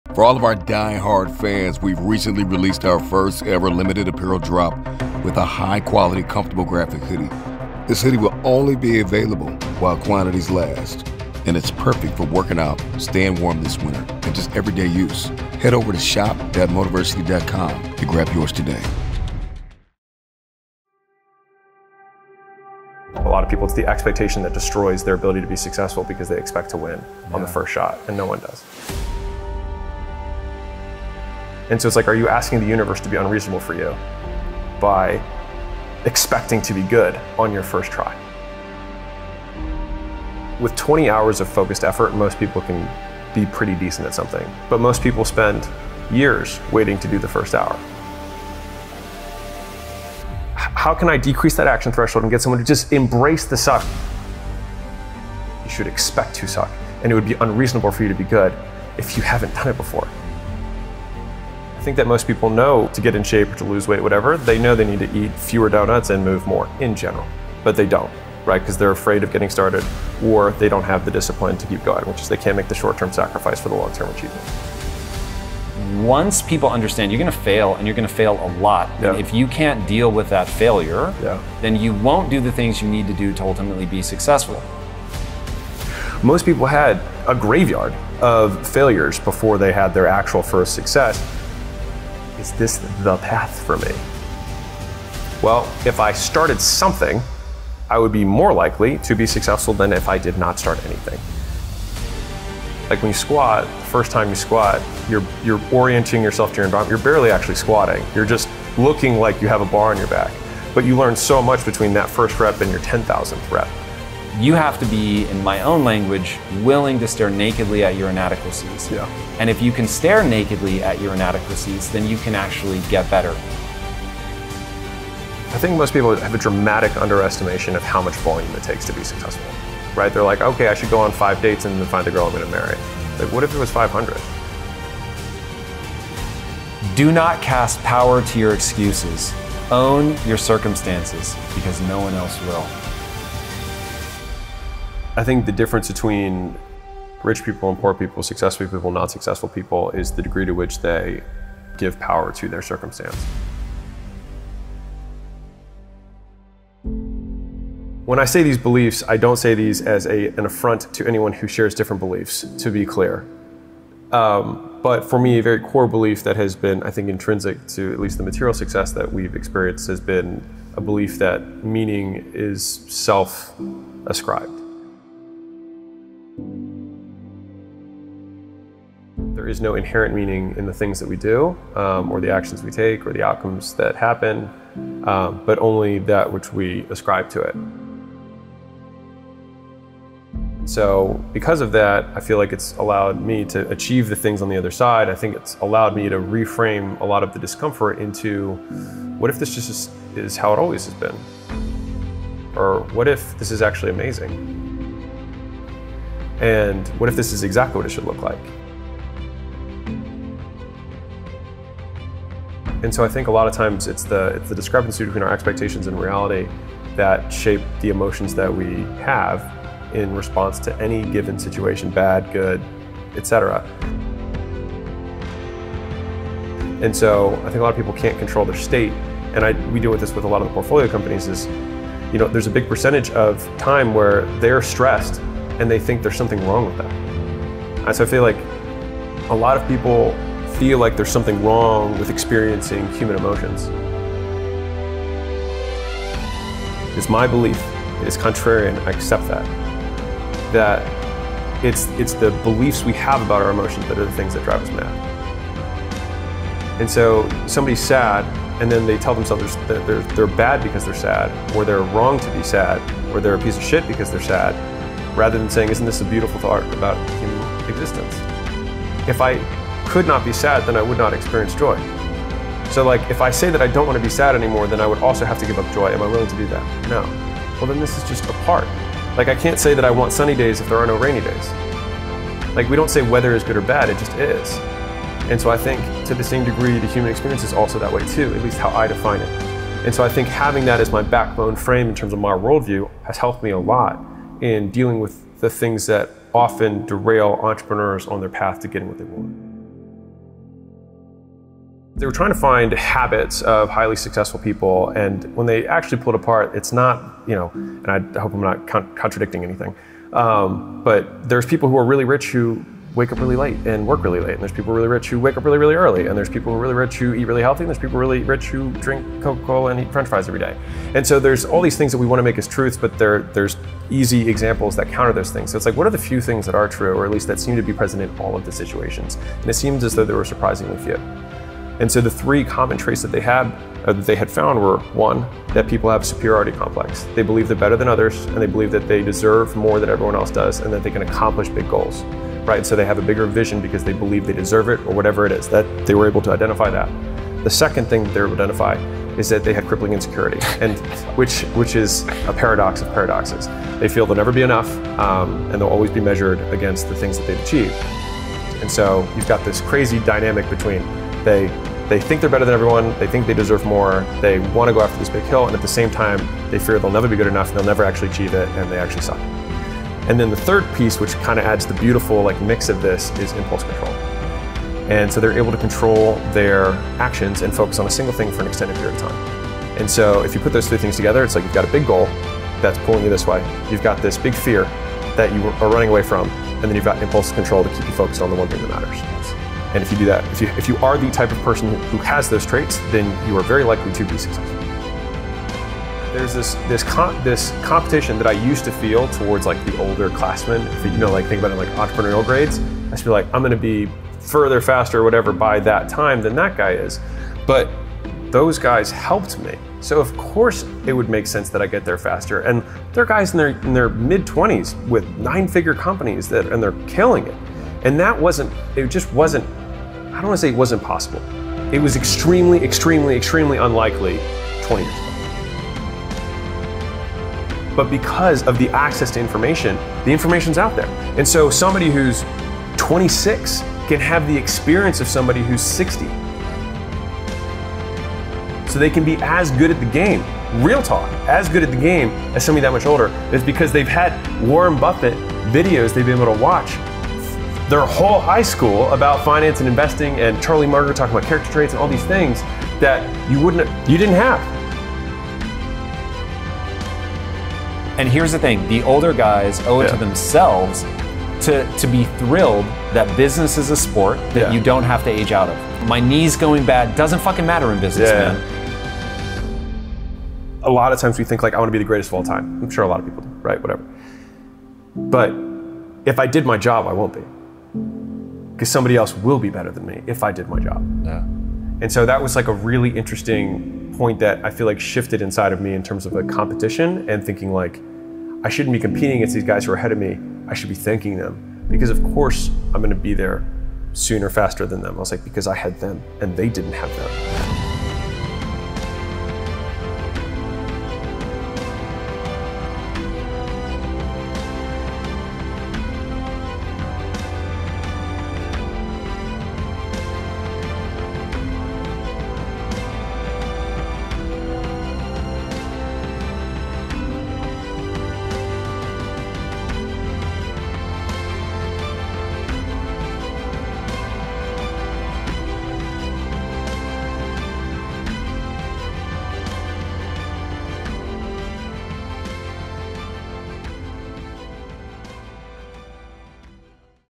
THIS IS WHY ONLY 1% SUCCEED | An Eye Opening Interview with Alex Hormozi
"This is what the 1% won't tell you!" ALEX HORMOZI. How to build a $100M business!
►$100M Offers book
►$100M Offers Audiobook

💪New Motiversity "Great Things Take Time" Hoodie is now out - Limited Number available

Alex Hormozi, Founder of Acquisition.com, Gym Launch and Author of $100M Offers, delivers one of the best motivational speeches you will ever hear.

"“Building a business is risky. Living paycheck to paycheck is risky.
Some risks come with upside. Others do not." - Alex Hormozi

►Speaker

Alex Hormozi

Alex Hormozi is a first-generation Iranian-American entrepreneur, investor, and philanthropist. In 2013, he started his first brick & mortar business. Within three years, he successfully scaled his business to six locations. He then sold his locations to transition to the turnaround business. From there he spent two years turning 32+ brick & mortar businesses around using the same model that made his privately owned locations successful. After that experience, he packaged his process into a licensing model which scaled to over 4000+ locations in 4 years. Over that same four-year period, he founded and scaled three other companies to $120M+ in cumulative sales across four different industries without taking on outside capital. He has scaled and exited 7 companies. His most notable exit was his majority sale of his licensing company for $46.2M in 2021.

▶Music